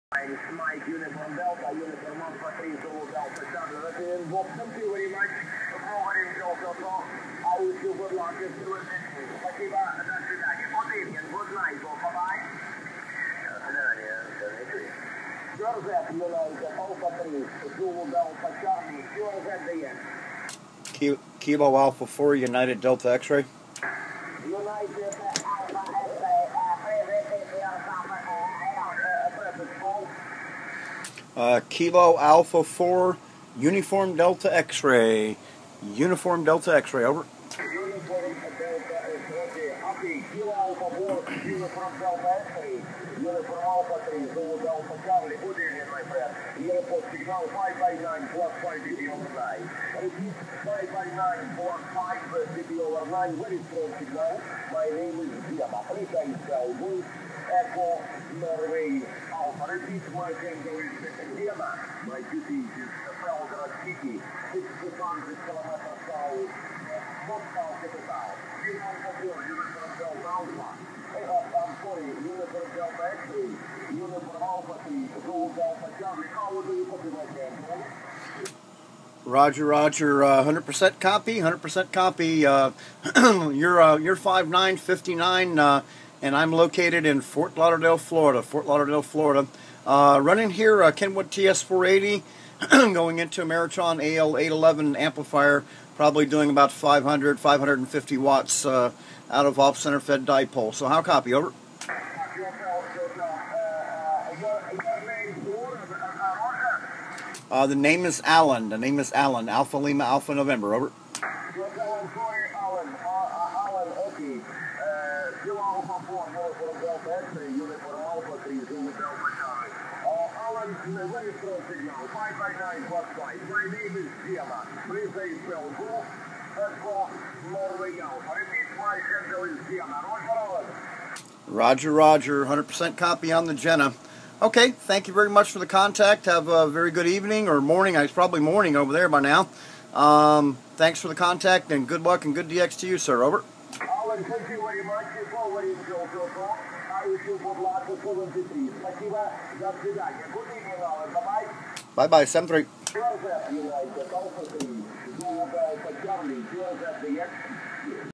Amateur Radio Contact - UA3ZDC - Russia
Nice 20 Meter contact with Gena - UA3ZDC in Russia